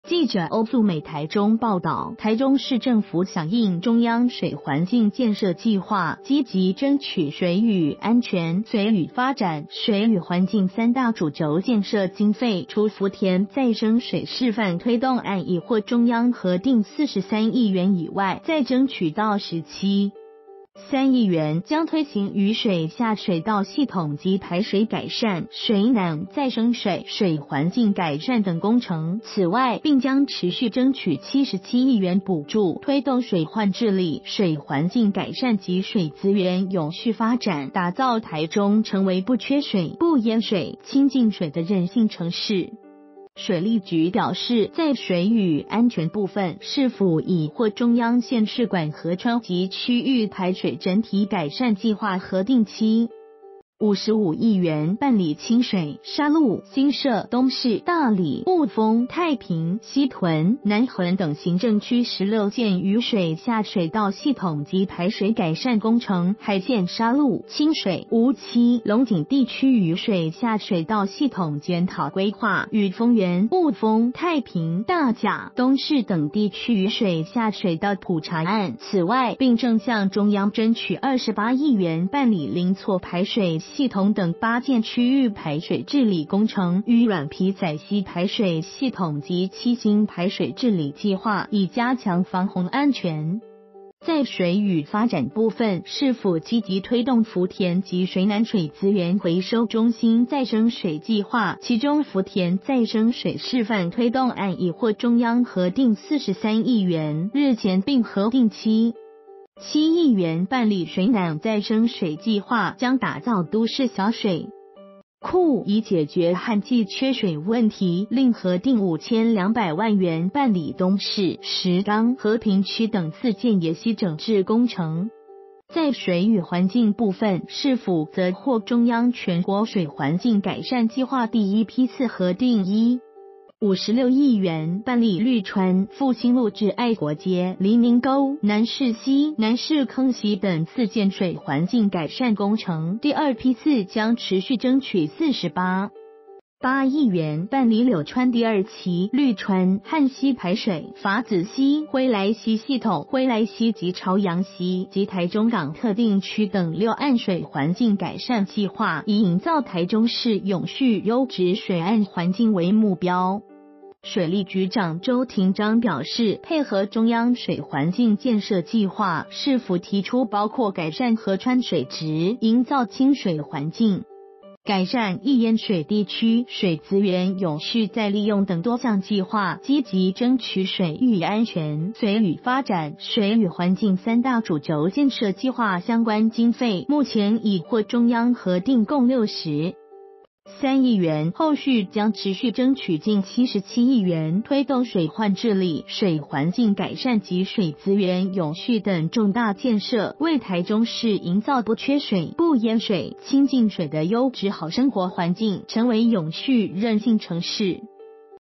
中市爭取60億元補助 打造親近水的韌性城市
〔記者歐素美／台中報導〕台中市政府響應中央水環境建設計畫，積極爭取「水與安全」、「水與發展」、「水與環境」3大主軸建設經費，除福田再生水示範推動案已獲中央核定43億元以外，再爭取到17.3億元，將推行雨水下水道系統及排水改善、水湳再生水、水環境改善等工程。此外，並將持續爭取77億元補助，推動水患治理、水環境改善及水資源永續發展，打造台中成為不缺水、不淹水、親近水的韌性城市。